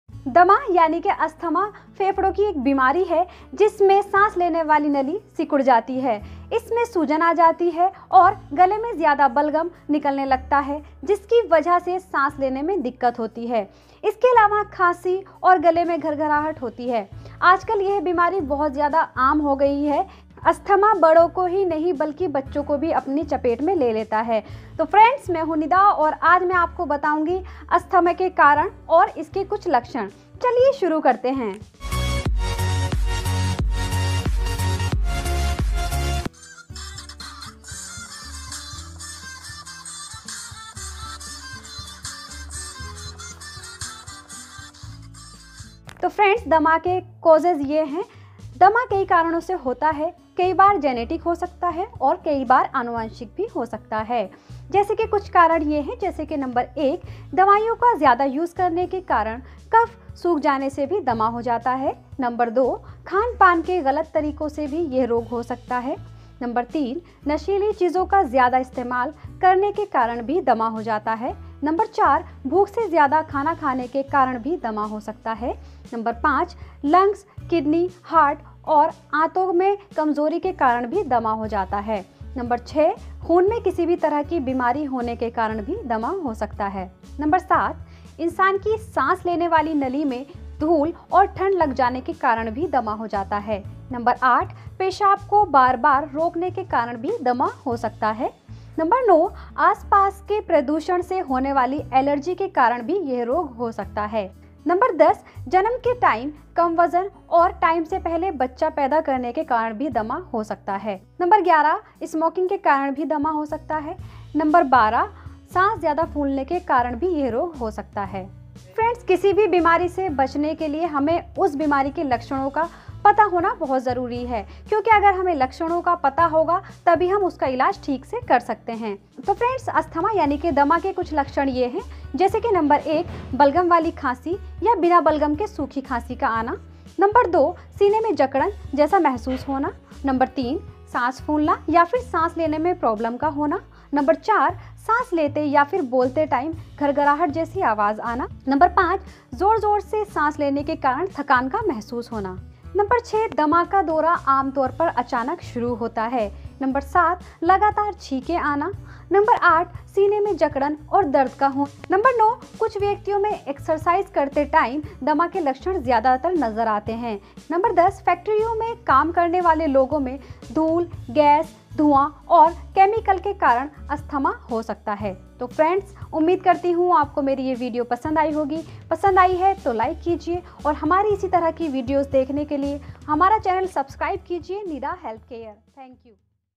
अस्थमा के लक्षण ओर कारण | Symptoms of Asthma |asthmaklakshn|asthmastmptomsinhindi| By Nida Ali
Asthama (Dma)
When the airways react to asthma triggers, people can experience what's called an asthma flare-up or asthma attack. Symptoms of an asthma attack include: coughing, chest tightness, wheezing and trouble breathing, according to the Centers for Disease Control and Prevention.